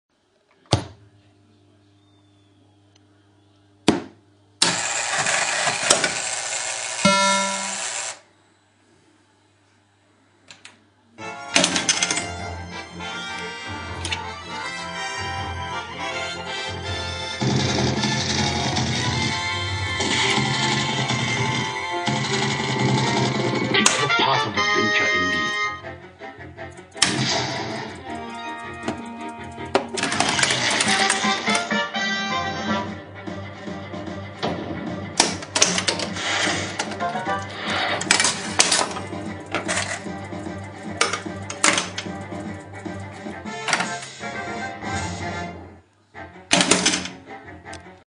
Indiana Jones Path of Adventure LED Mod In Machine
Now installed in my pinball machine. Hand activating the ADVENTURE targets to start the mode...